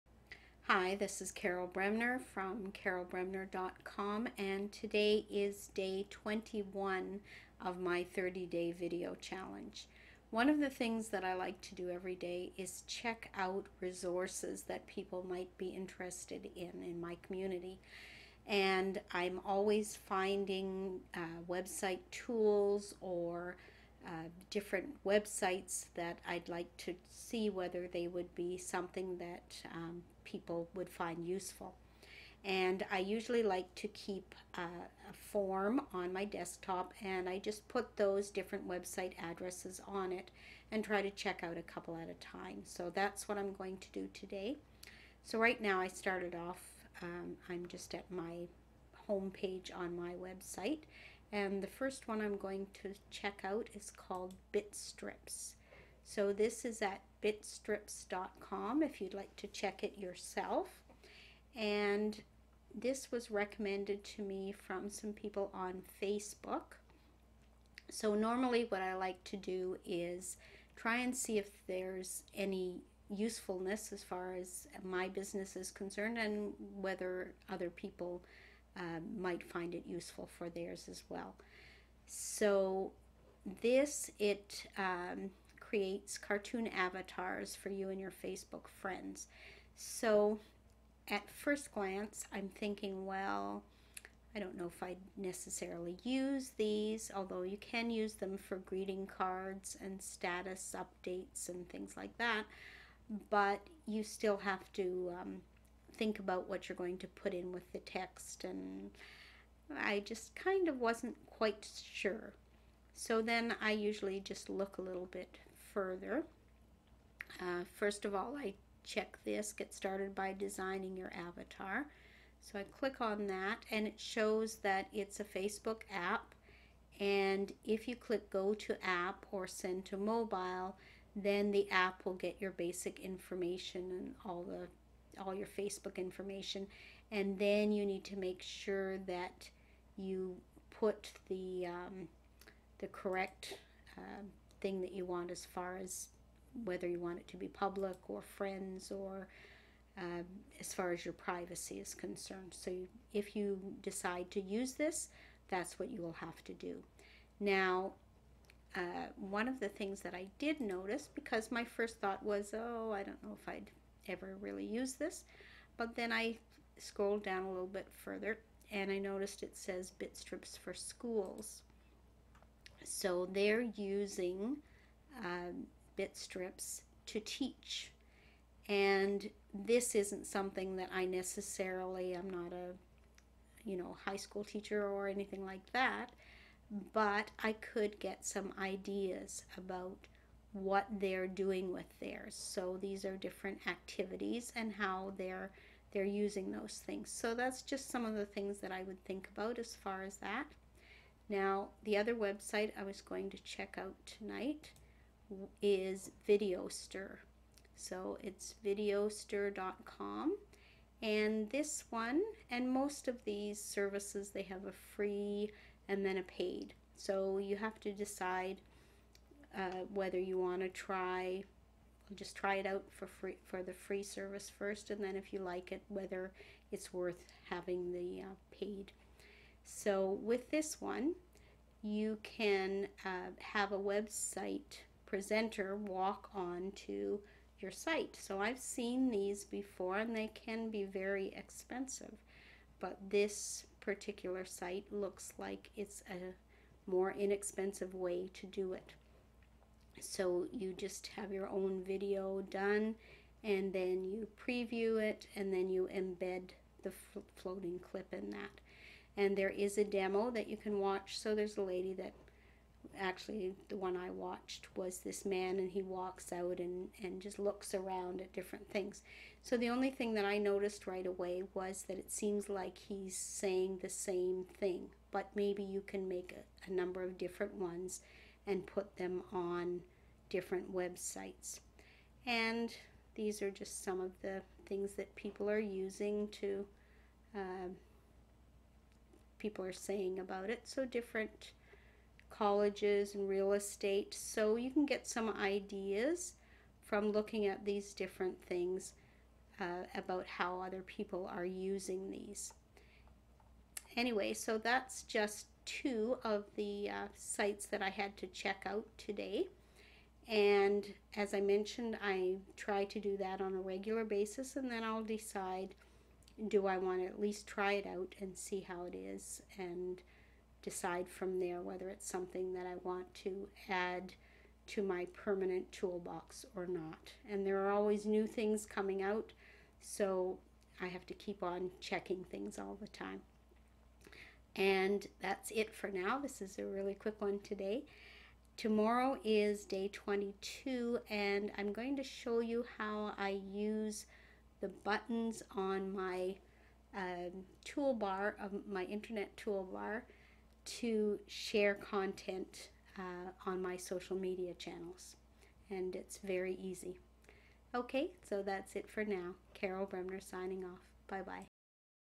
Day 21 New Social Media Tools
Part of online business involves research, so today I'm checking out two online marketing tools that might be useful to use. One is for Facebook and the other for a website.